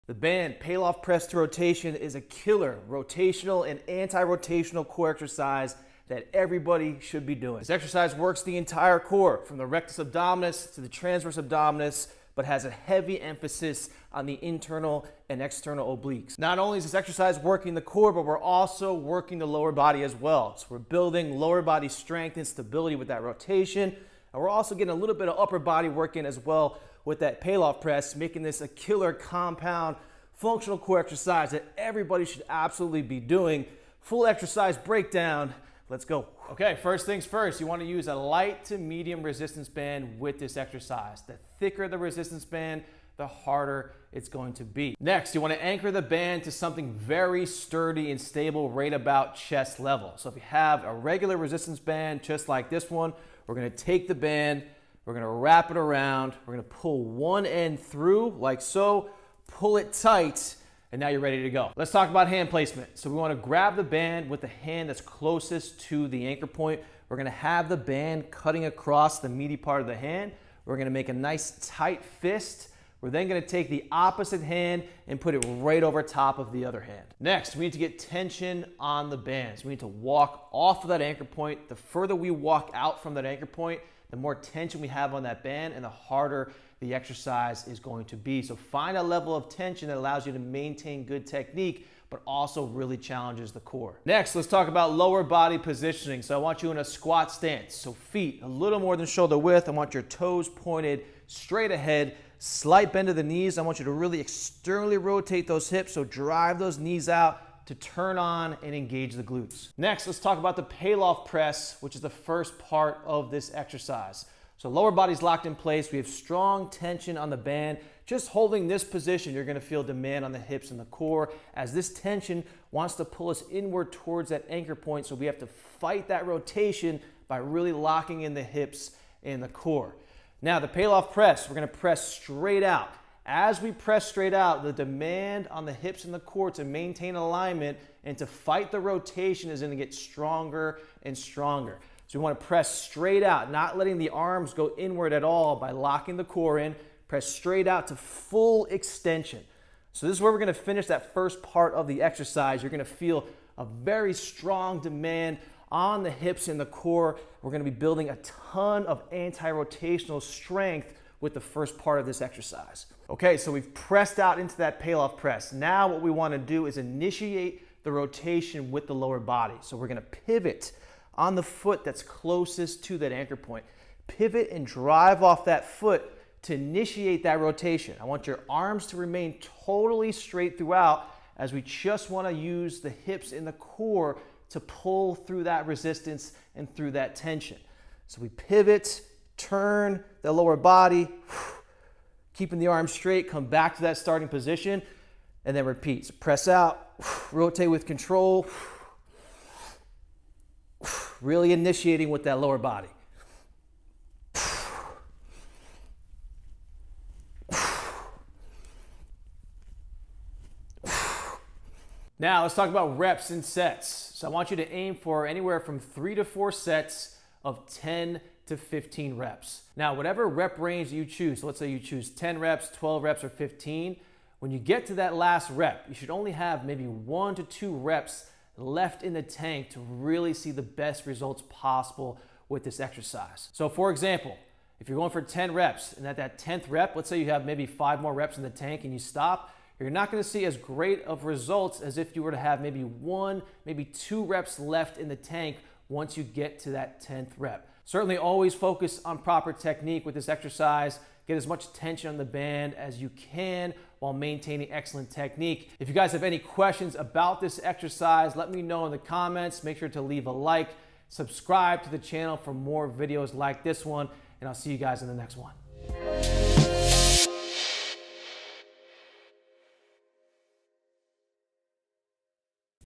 The Core Exercise YOU SHOULD BE DOING
The Band Paloff Press to Rotation is a killer anti rotational / rotational core exercise that everyone should be doing! Grab a band and get to work!